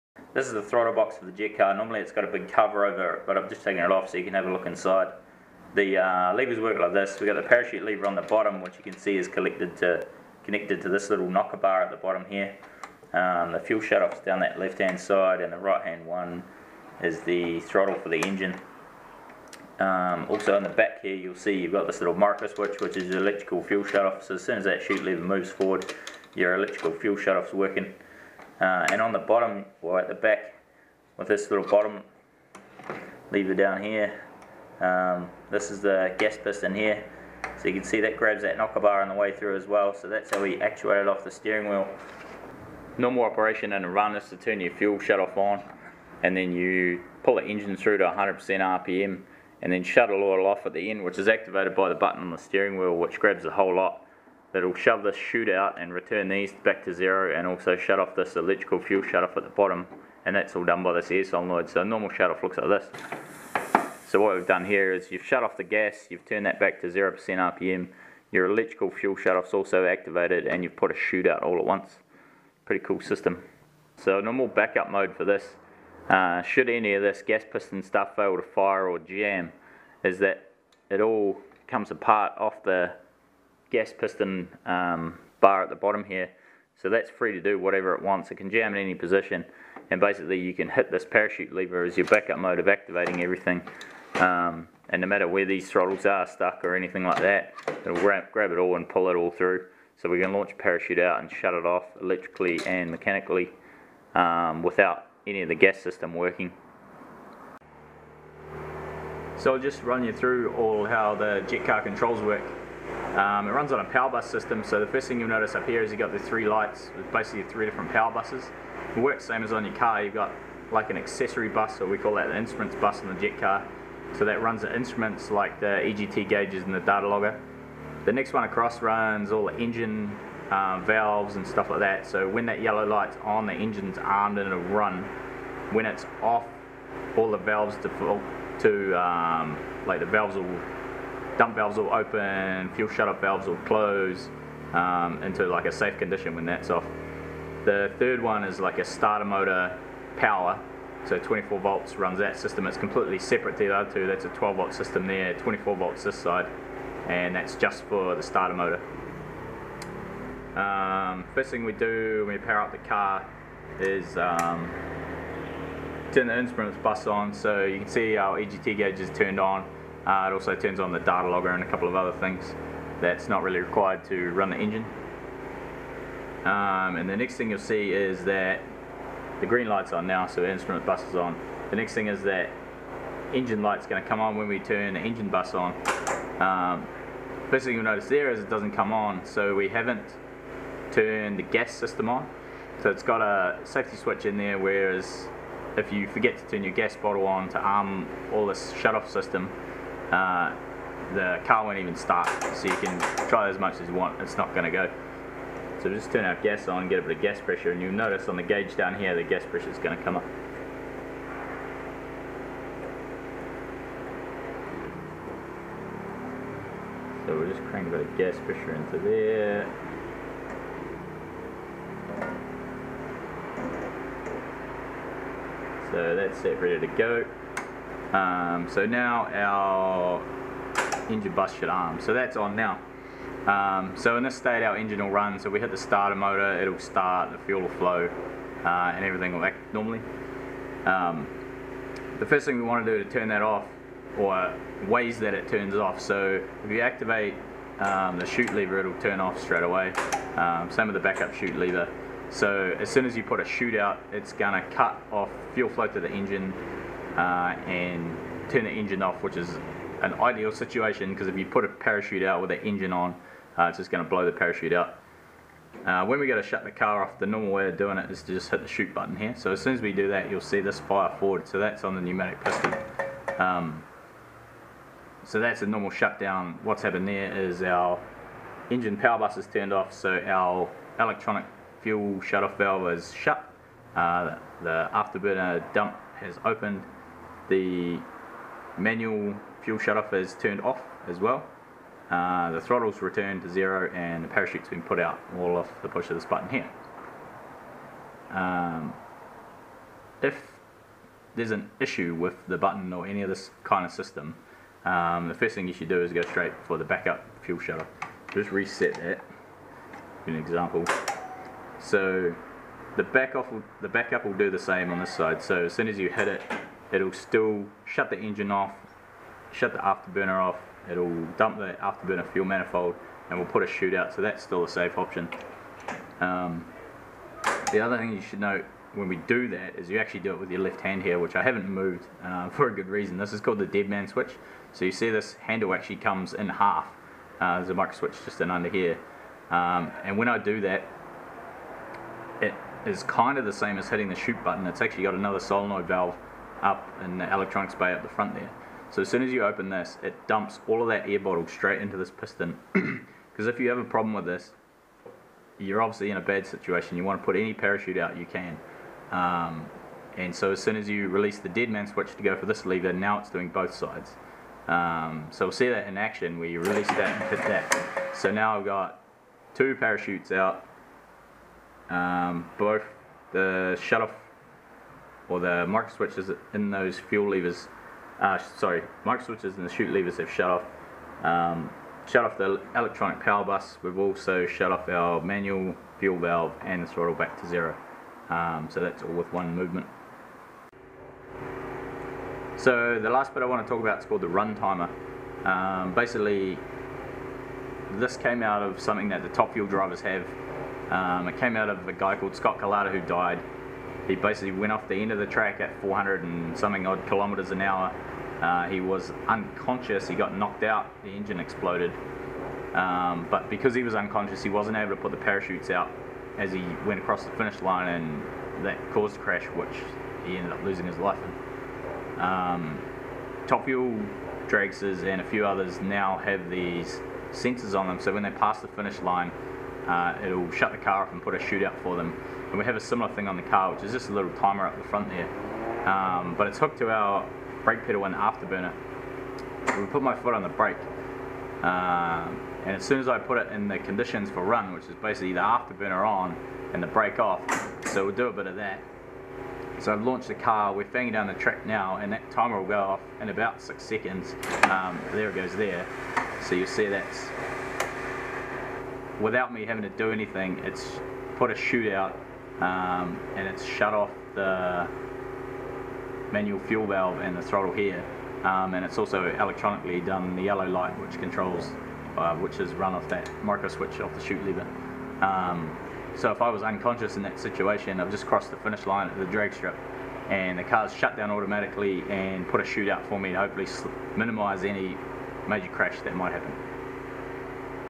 Throttle system
The parachute lever and fuel shutoff system being tested on the jet dragster. Every component has a backup in case of failure, even the driver!